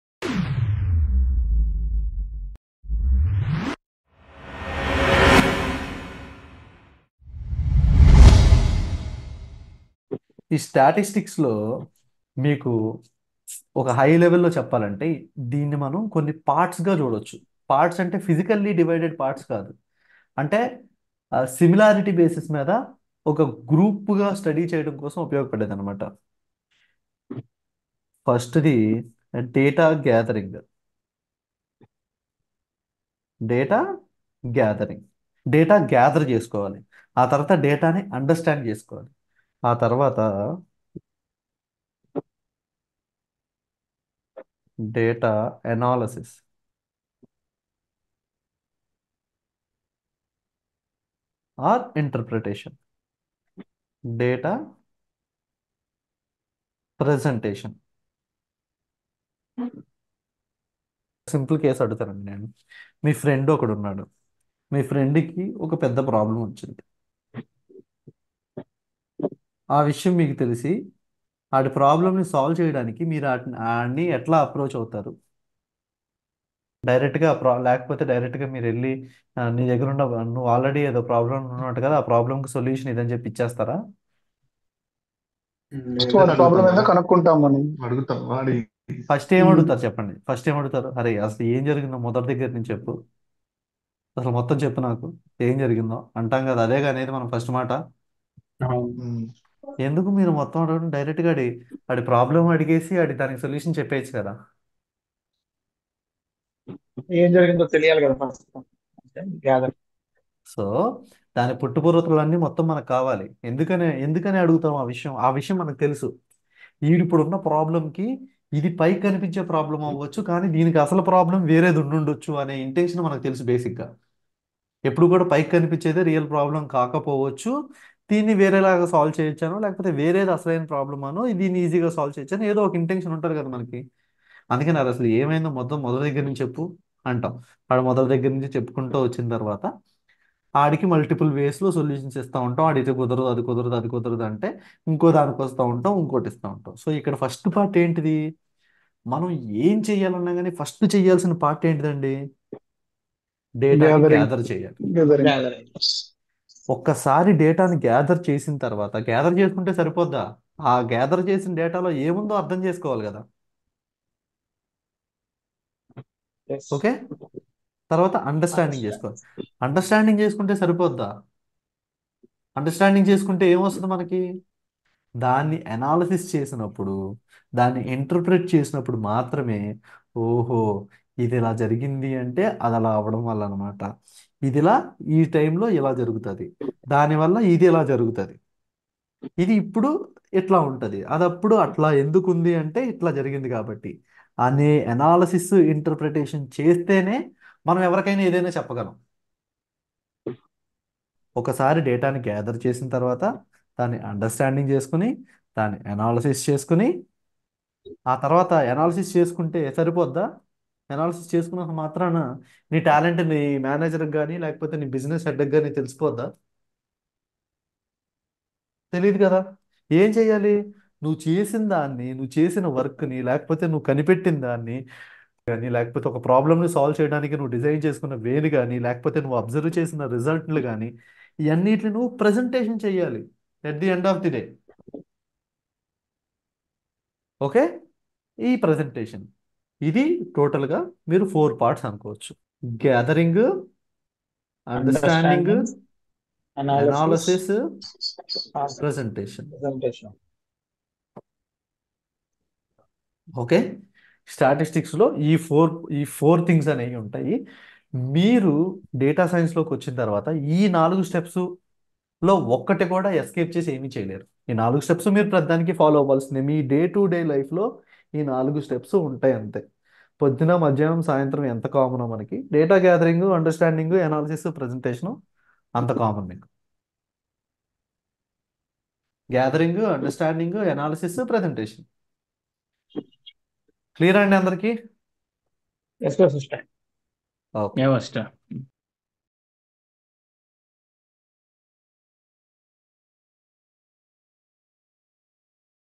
Lecture 2 : Types Of Statistics in Telugu
In this video, we'll explore the 4 steps in statistical analysis in Telugu. Whether you're new to statistics or looking to enhance your skills, this video is perfect for you. We'll cover data collection, analysis, interpretation, and presentation.

4 steps in statistical analysis in telugu,
what are the 4 steps in statistical analysis in telugu,
telugu statistical analysis secrets.